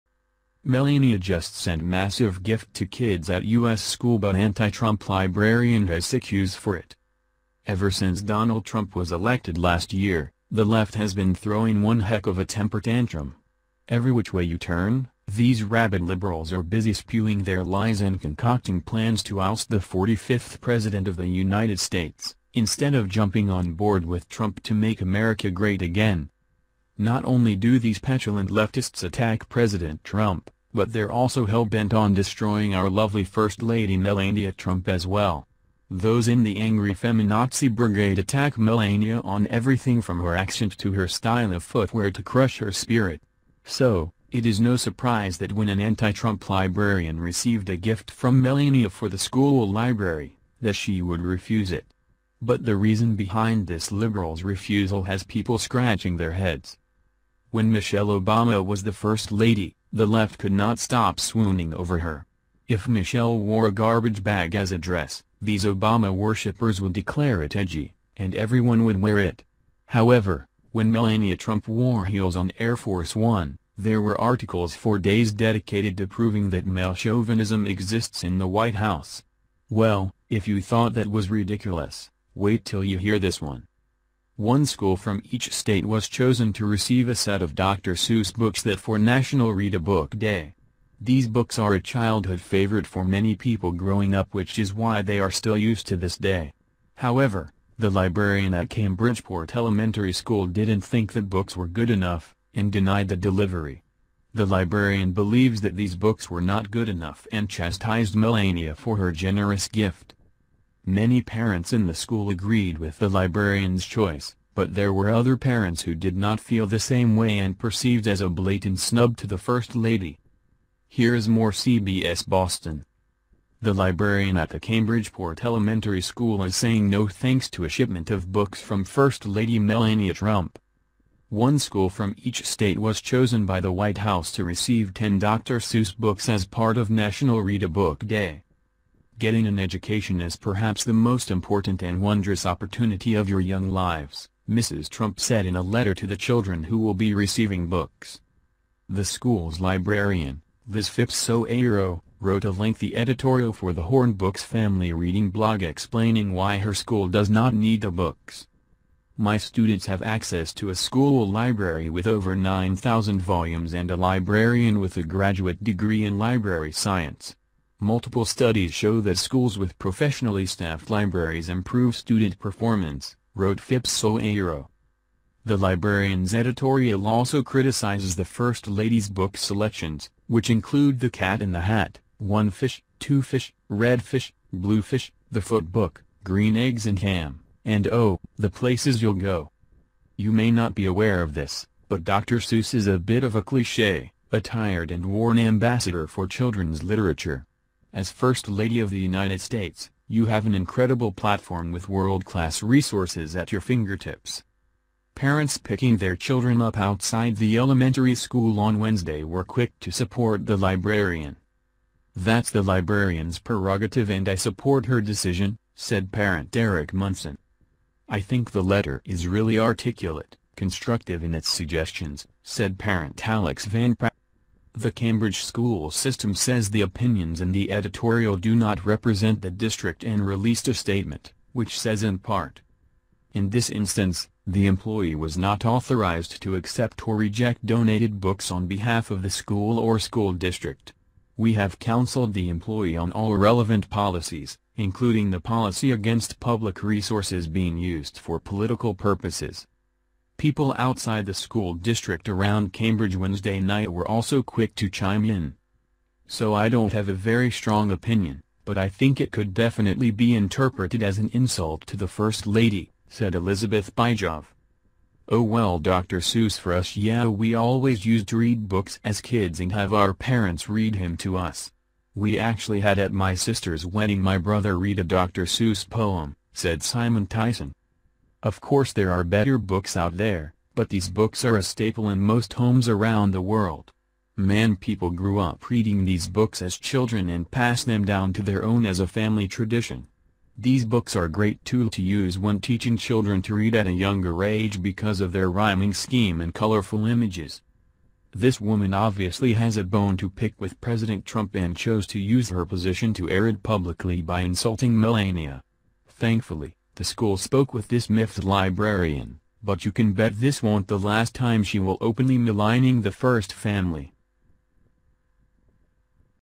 Melania Just Sent Massive Gift To Kids At U S School But Anti Trump Librarian Has Sick Use For It
Melania Just Sent Massive Gift To Kids At U S  School But Anti Trump Librarian Has Sick Use For It - Fast News 24h